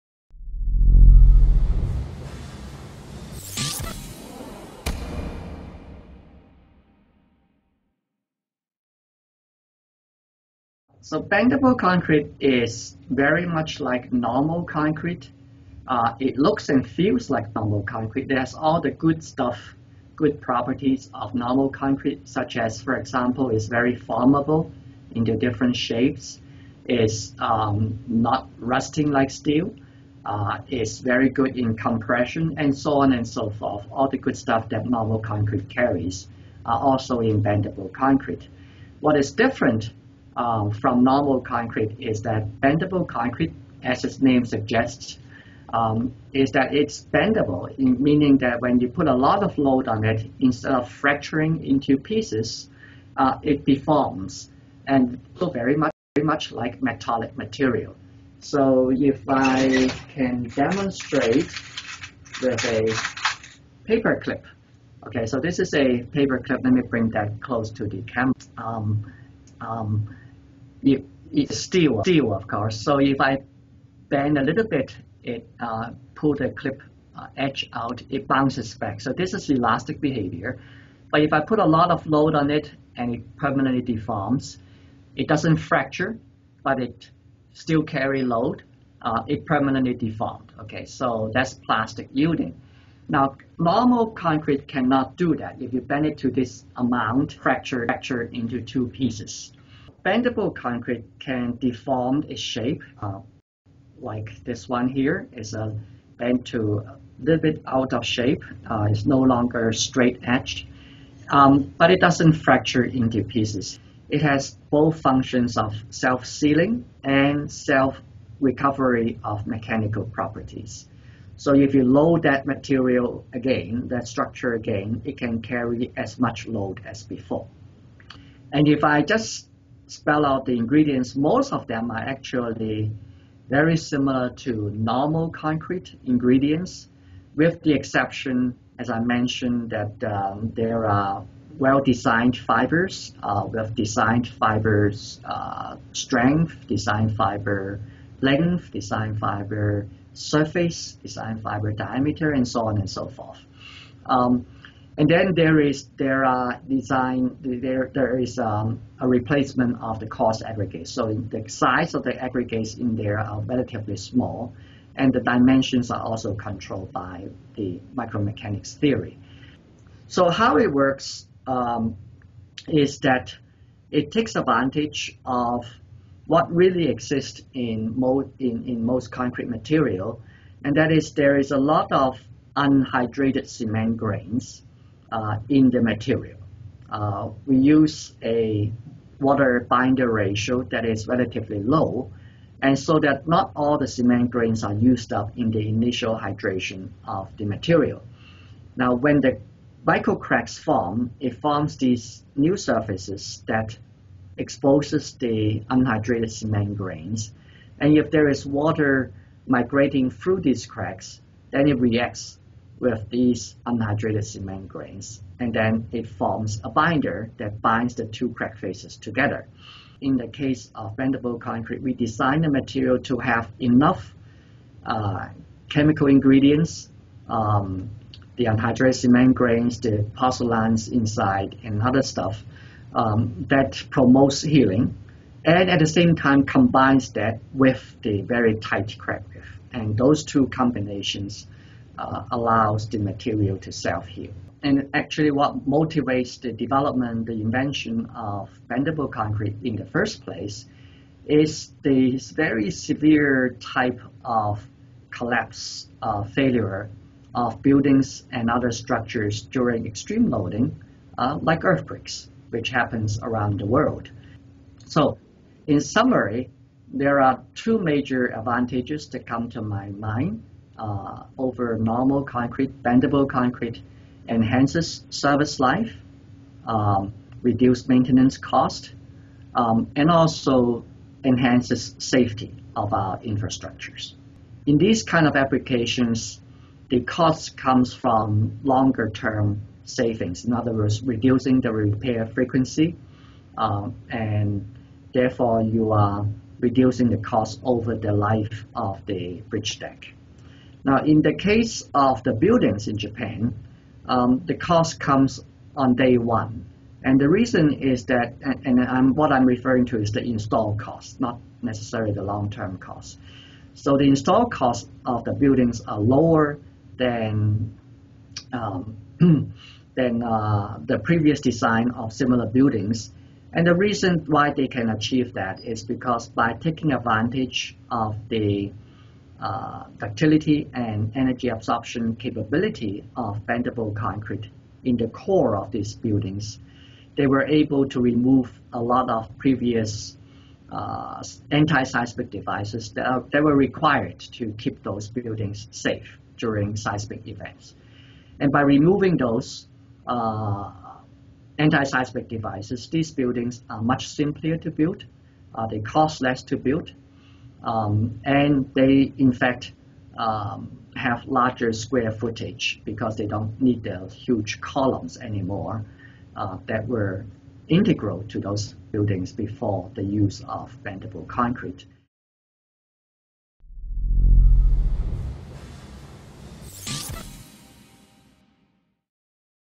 Bendable concrete: A solution to high maintenance costs?
Professor Victor Li from the University of Michigan designed a "smarter" concrete that may eliminate the need for long-term infrastructure maintenance. Bendable concrete is not only stronger but is additionally able to heal its own cracks.